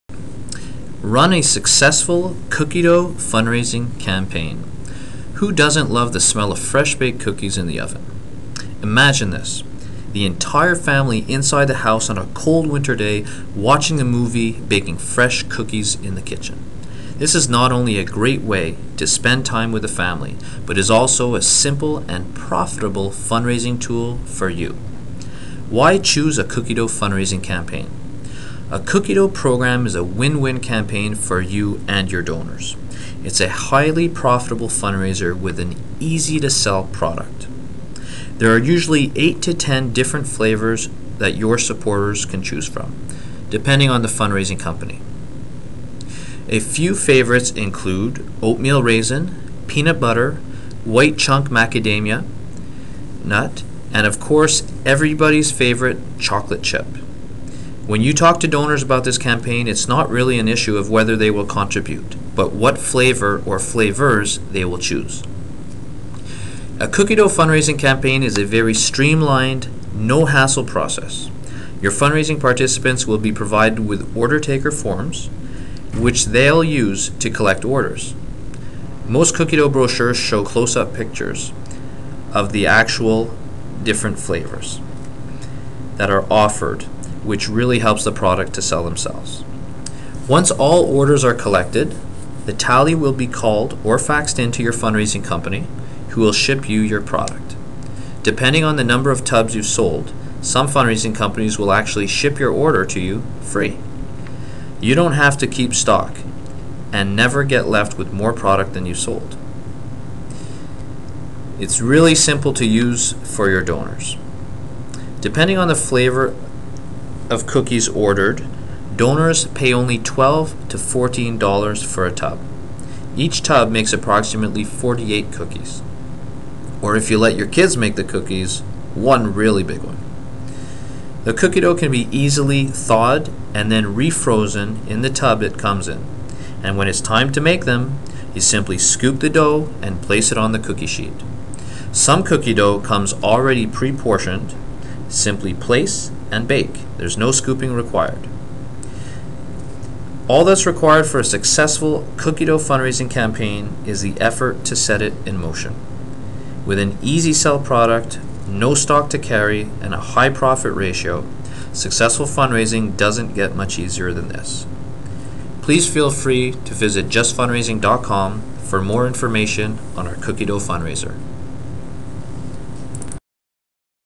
Run a Successful Cookie Dough Fundraising Campaign
Who doesnt love the smell of fresh baked cookies in the oven? This is a very simple and profitable fundraiser for you.